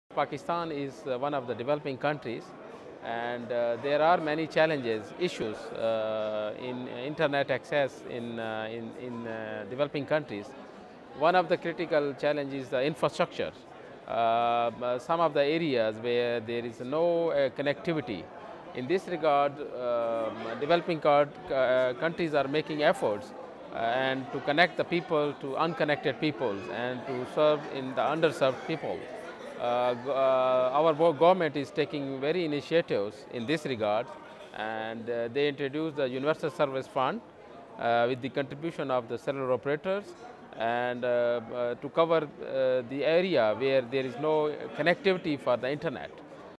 Global IGF 2017: Syed Iftikhar Shah on The Key Internet Issue
Syed Iftikhar Shah, software engineer and director of IT for Government of Pakistan, shares their answer to the question, “What is the most important Internet issue to address today for the best tomorrow?” Recorded at the Global Internet Governance Forum 2017 in Geneva, Switzerland, by the Imagining the Internet Center, an initiative of Elon University, North Carolina, USA.